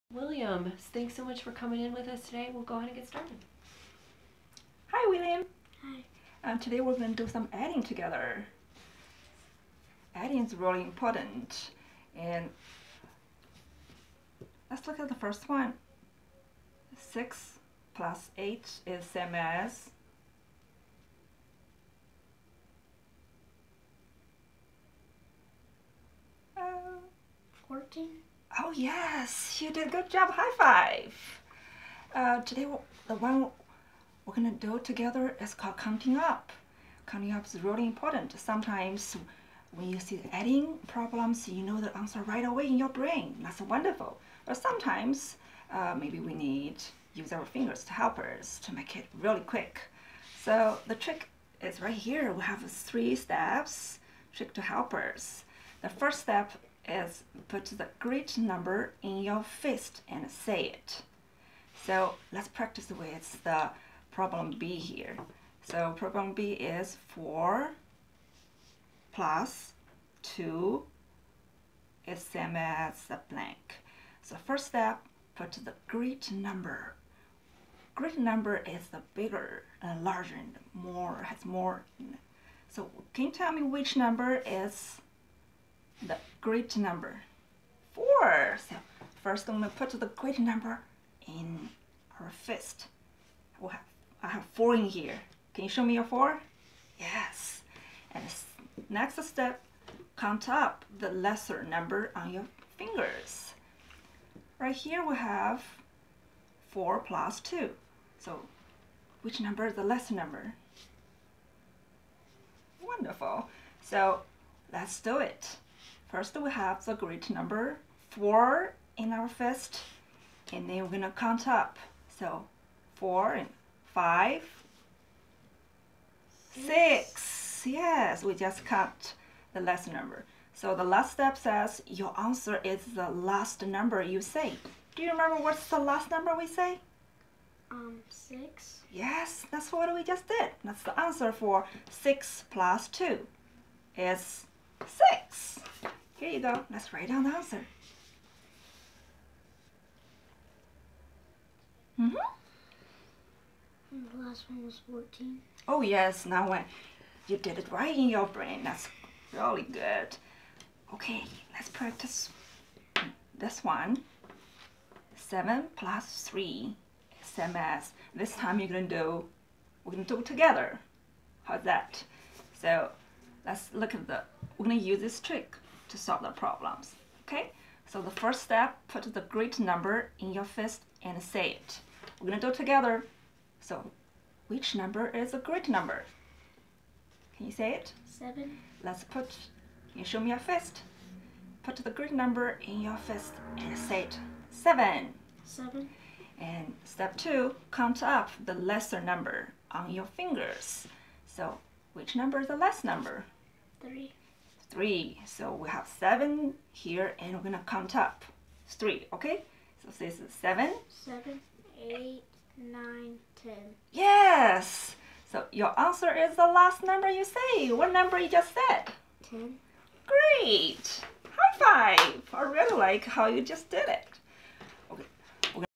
Video Example: A tutor and student practice adding using the counting up strategy
In this example, a tutor and student practice adding using the counting up strategy. It is associated with Module 8 Activity 3